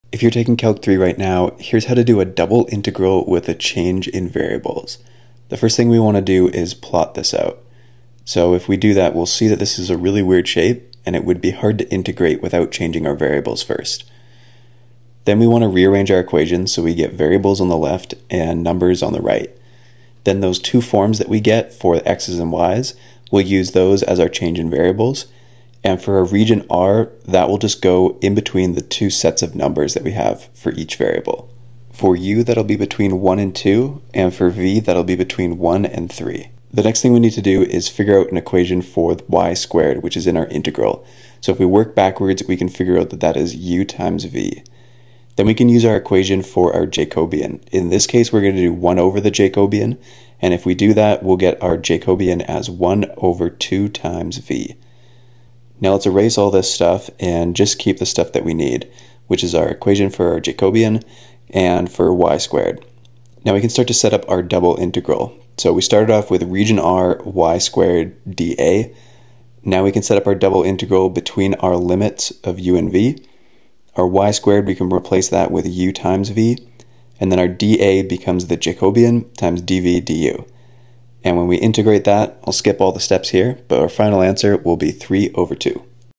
Double Integrals Change in Variables
For the calc 3 people: here’s how to do a double integral with a change in variables.

These questions usually show up near the end of calc 3 as part of the multivariable integration section.

The change in variables takes our weirdly shaped region R and changes in the a rectangular region in u and v.